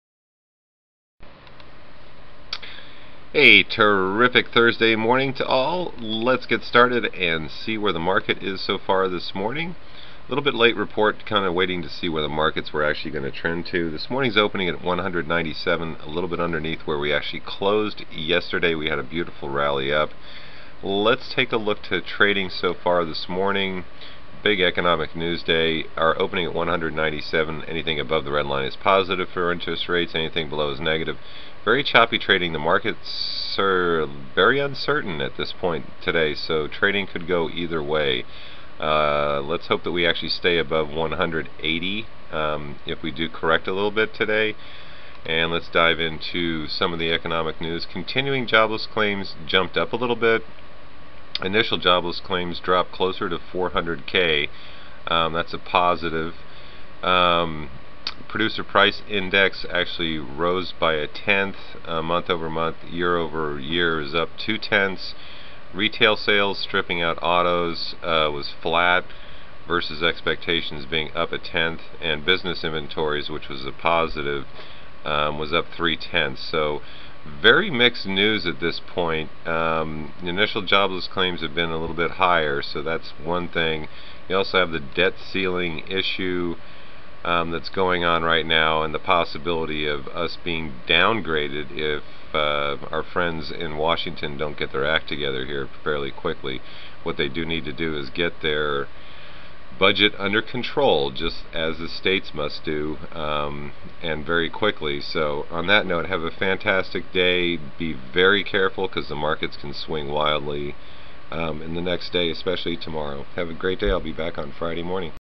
BSR-2011.07.14-09.00.36.avi
A Terrific Thursday Morning to all,
Morning Economic News: Initial Jobless Claims drop, Continuing Jobless Claims were slightly up, Retail sales flat, Business Inventories were up. Core PPI was up for the month and year.
Focus is on our debt ceiling and the possible downgrade unless solutions are agreed upon.
Be careful the markets can swing either way.
Have a great morning!